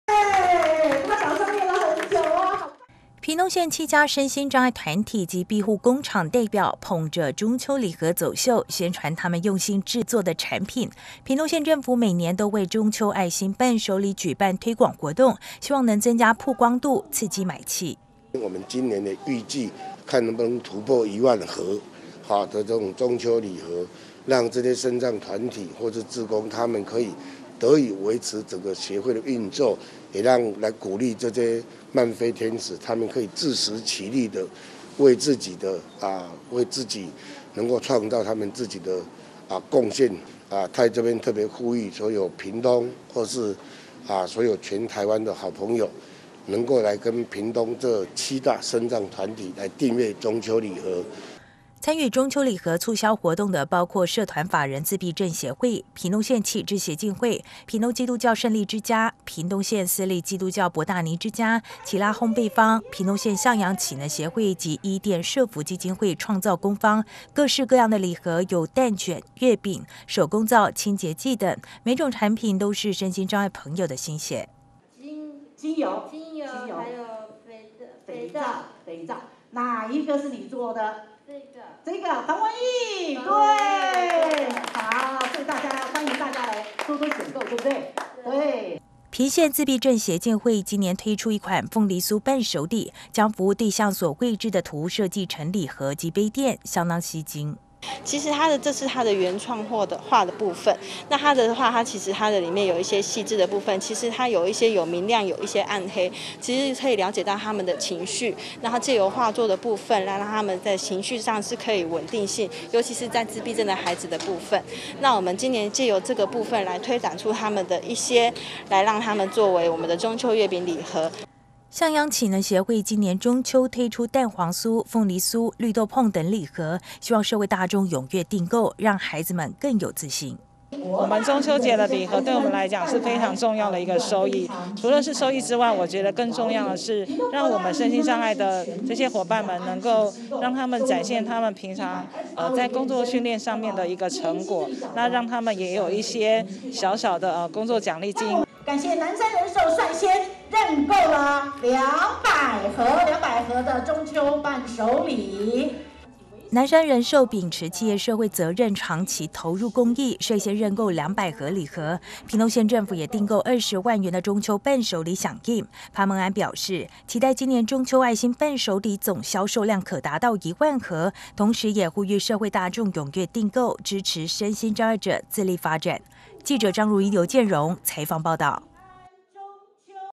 屏東市/社會關懷 111 0818 屏縣府促銷中秋愛心伴手禮 支持身心障礙者自立
中秋佳節將至，市面上琳瑯滿目的禮盒紛紛推出，為了支持身心障礙朋友自立生活，屏東縣政府在身心障礙福利服務中心舉行 身心障礙團體及庇護工場 中秋伴手禮促銷 活動，同時率先訂購20萬元的中秋伴手禮，縣長潘孟安也呼籲企業及社會大眾踴躍採購，給予身心障礙朋友最大的支持及鼓勵。